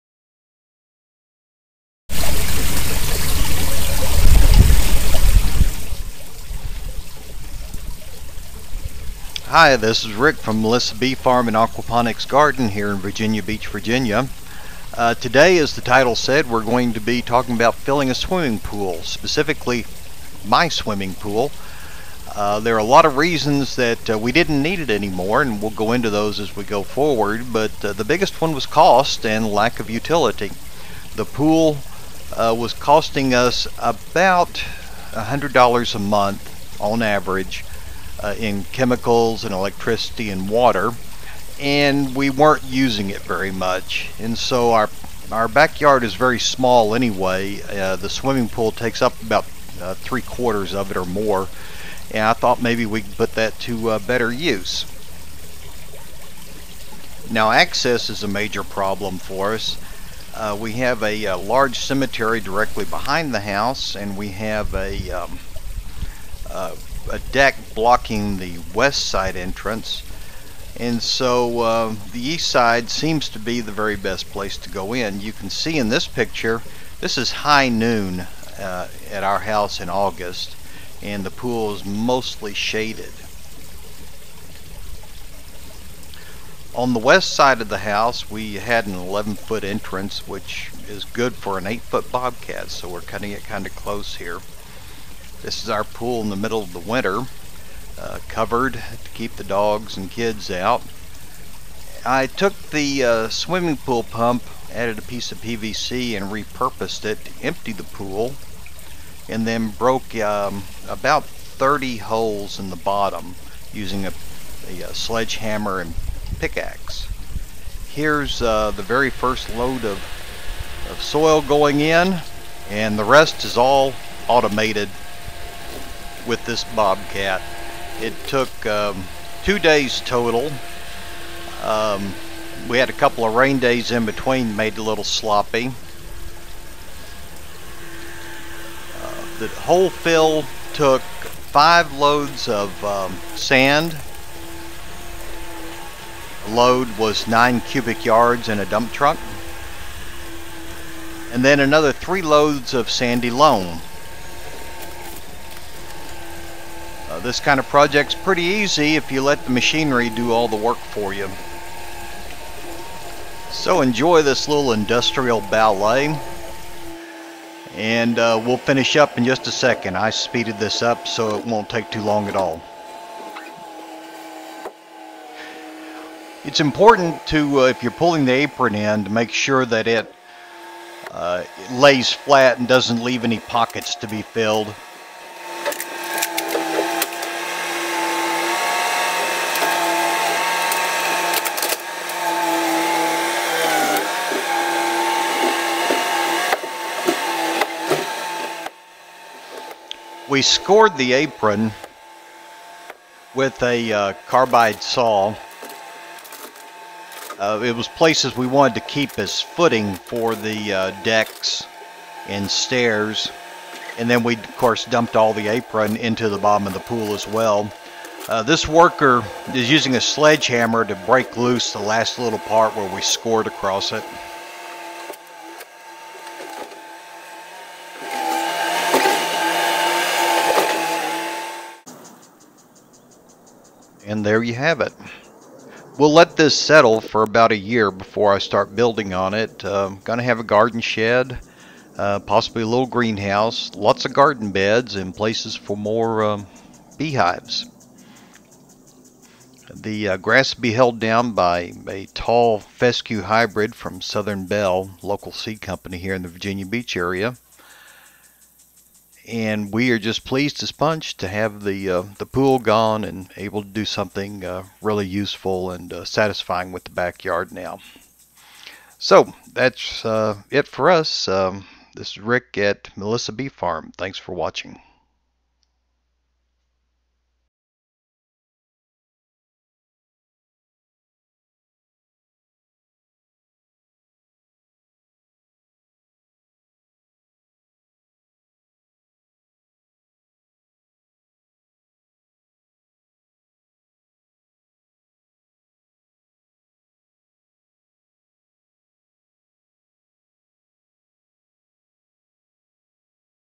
pool filling
How we turned a money drain into productive yard by filling our unwanted swimming pool.